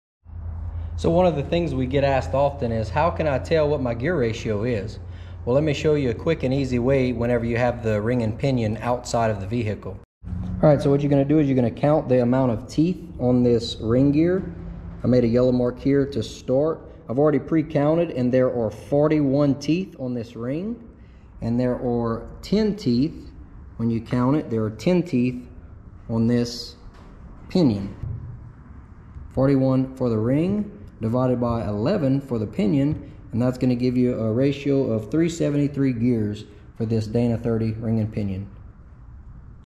What’s My Gear Ratio? 👈🏽
How do I figure out my gear ratio? Well one was is to take the number of teeth on the ring, and divide it by the number of teeth on the pinion. I explain in the shortvideo